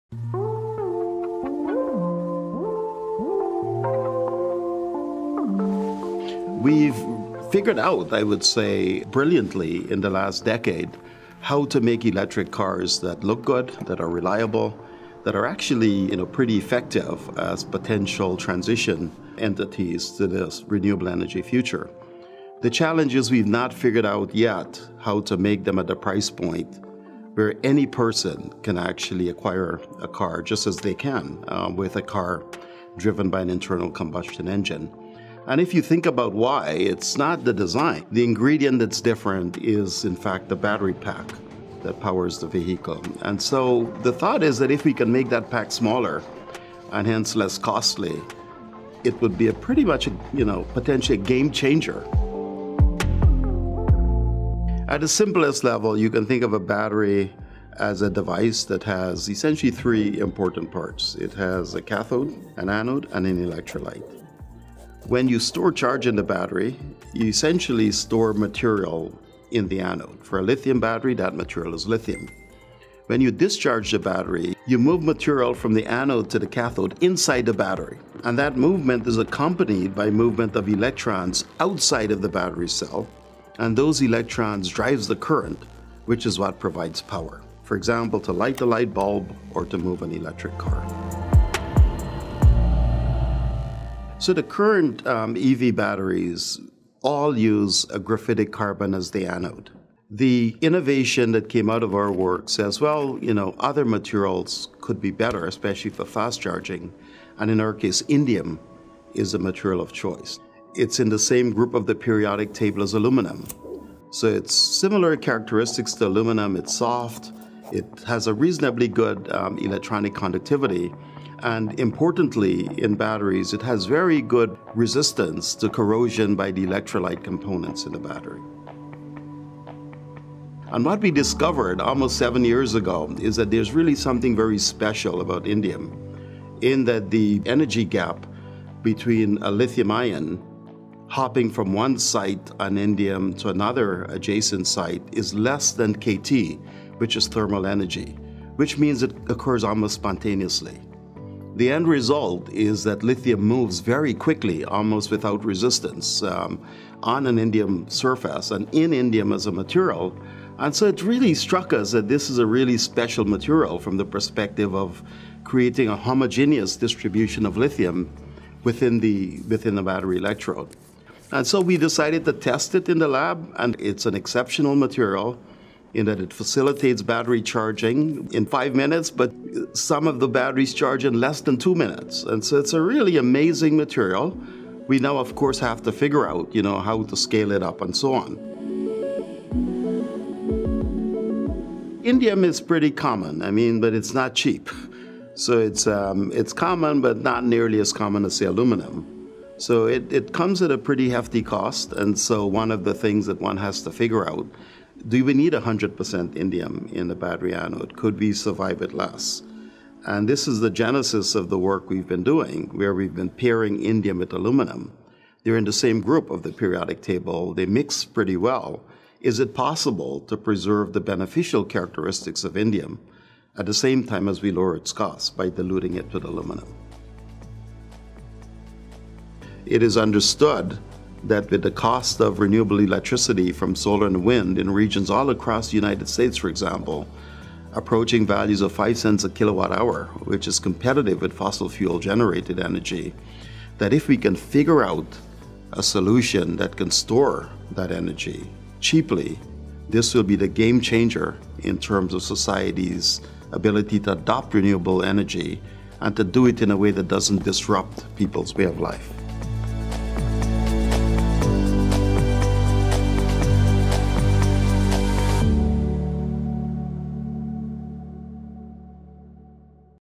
Can you charge an EV battery in the time it takes to watch this video?
A team in Cornell Engineering created a new lithium battery that can charge in under five minutes – faster than any such battery on the market – while maintaining stable performance over extended cycles of charging and discharging.

The breakthrough could alleviate “range anxiety” among drivers who worry electric vehicles cannot travel long distances without a time-consuming recharge.